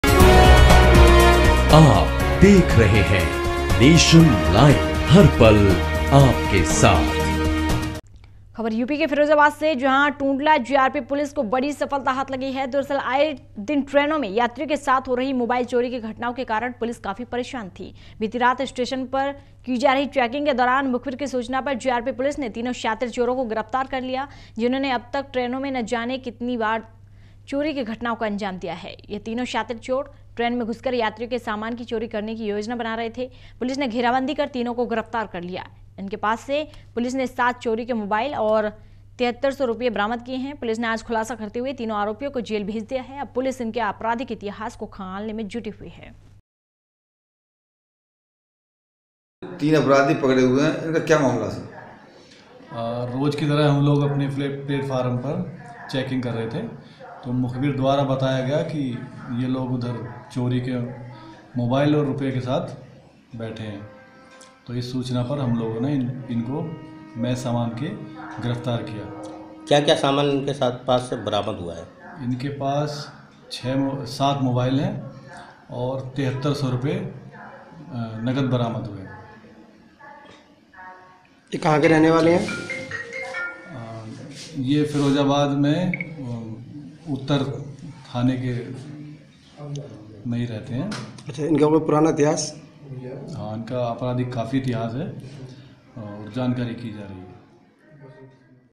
Nation Live_फिरोज़ाबाद- टूण्डला GRP पुलिस को मिली बड़ी सफलता
Nation live 24*7 Hindi NEWS channel
यूपी के फिरोज़ाबाद से, जहां टूण्डला जीआरपी पुलिस को बड़ी सफलता हाथ लगी है...दरअसल, आए दिन ट्रेनों में यात्रियों के साथ हो रही मोबाइल चोरी की घटनाओं के कारण पुलिस काफी परेशान थी. बीती रात स्टेशन पर की जा रही चेकिंग के दौरान मुखबिर की सूचना पर जीआरपी पुलिस ने तीनों शातिर चोरों को गिरफ्तर कर लिया. जिन्होंने अब तक ट्रेनों में न जाने कितनी बार चोरी की घटनाओं को अंजाम दिया है...यह तीनों शातिर चोर ट्रेन में घुसकर यात्रियों के सामान की चोरी करने की योजना बना रहे थे. पुलिस ने घेराबंदी कर तीनों को गिरफ्तार कर लिया. इसके पास से पुलिस ने 7 चोरी के मोबाइल और 7300 रुपये बरामद किए है...पुलिस ने आज खुलासा करते हुए तीनो आरोपियों को जेल भेज दिया है...अब पुलिस इनके अपराधिक इतिहास को खंगालने में जुटी हुई है...।